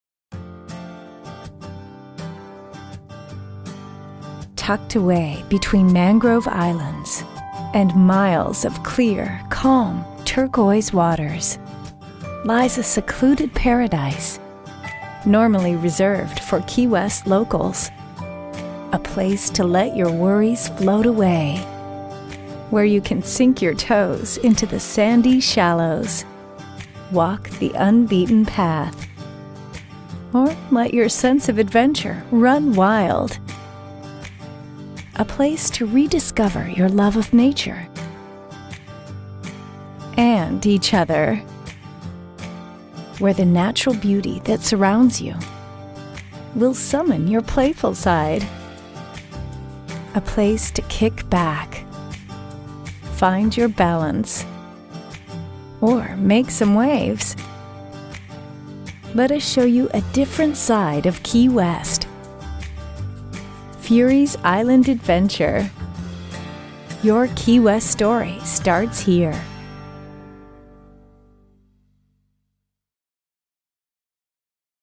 Key West Watersports | Fury Island Adventure Eco Tour
Find a secluded island paradise in the Key West backcountry with Fury's Island Adventure Eco Tour. Relax in the warm, shallow, aqua waters while snorkeling and kayaking with friends. Let your worries float away and rediscover your love of nature.